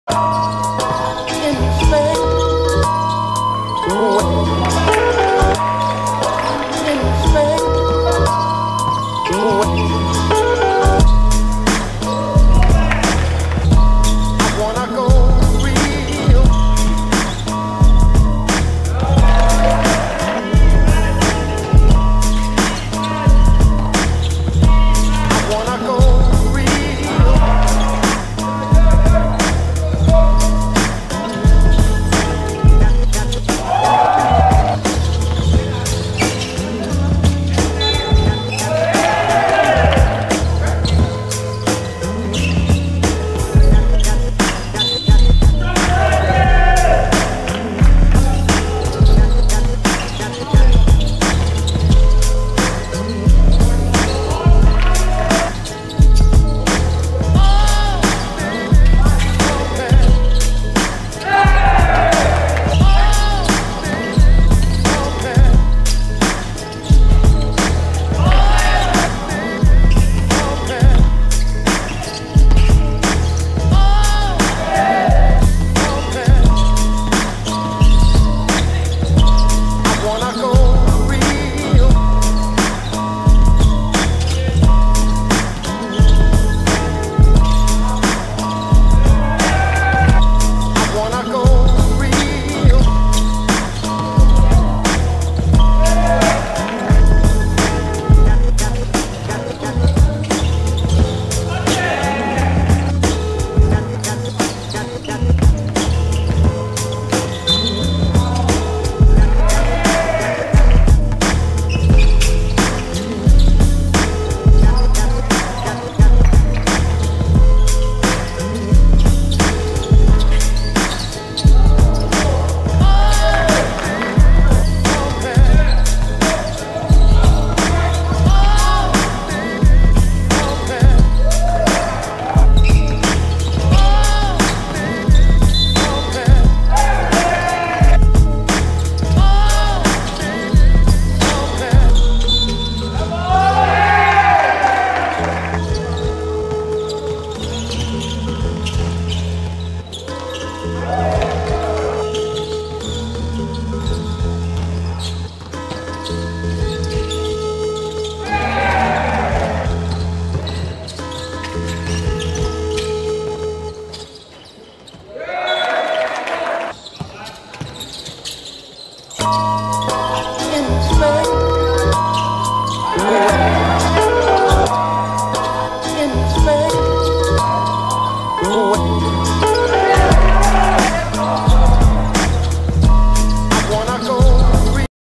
15 07 Chicago R vs Brooklyn R – Draft Camp 63
CHICAGO R 56:61 BROOKLYN R
M.Jamorski 24pkt
T.Stępień 15pkt 17zb